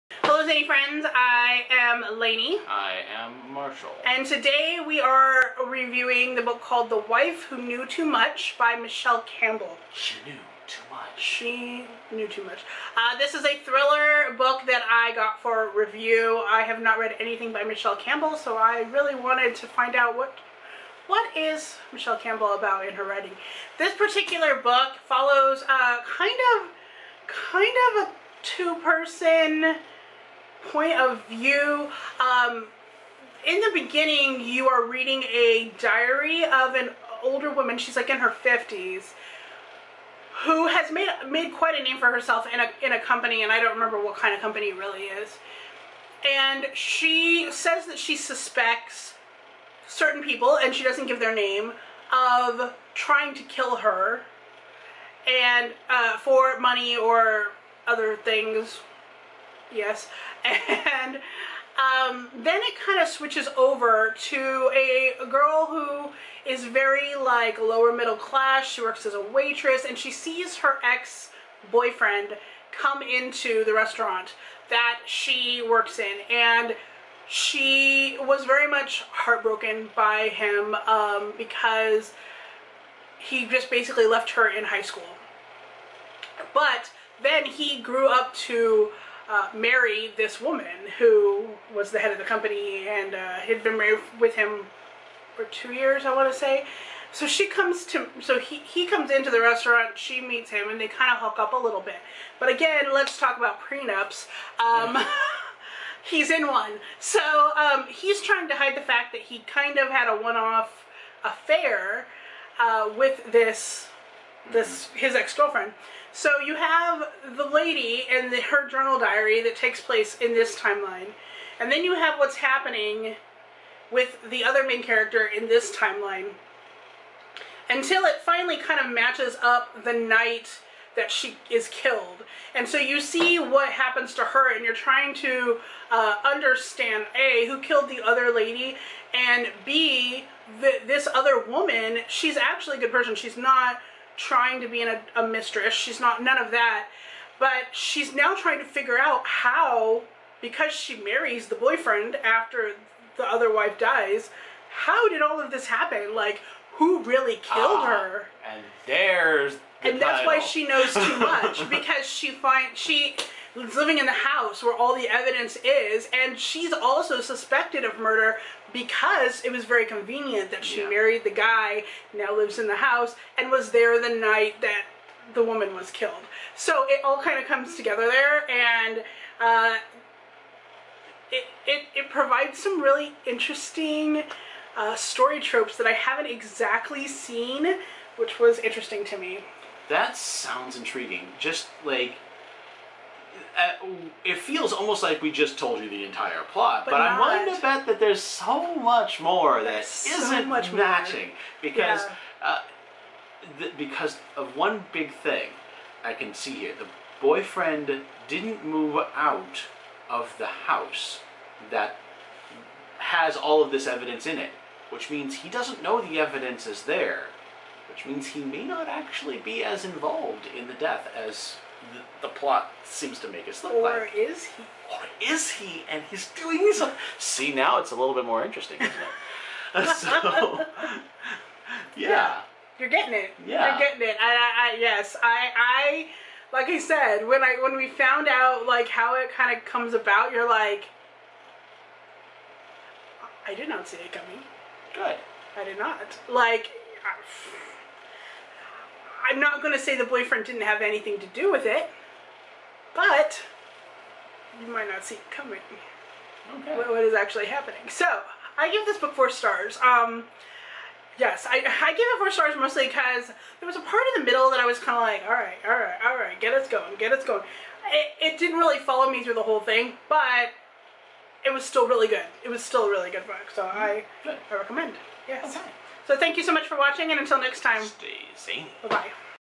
Book Review: The Wife Who Knew Too Much | Michele Campbell
📖 BOOK REVIEW⠀📚
BOOK: The Wife Who Knew Too Much
AUTHOR: Michele Campbell
Stars: ⭐⭐⭐⭐
Published: July 28, 2020